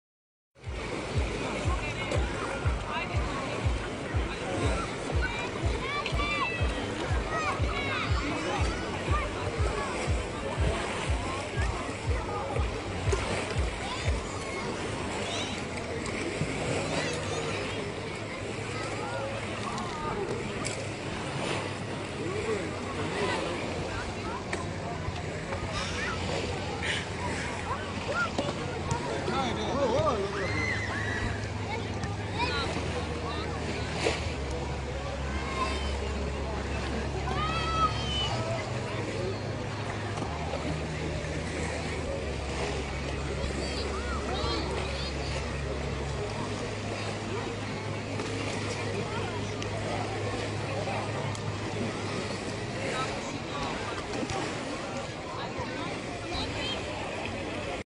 Los amos del frisbee
Estos dos son los p. amos del frisbee. Que arte tienen agunos y que poca cabeza!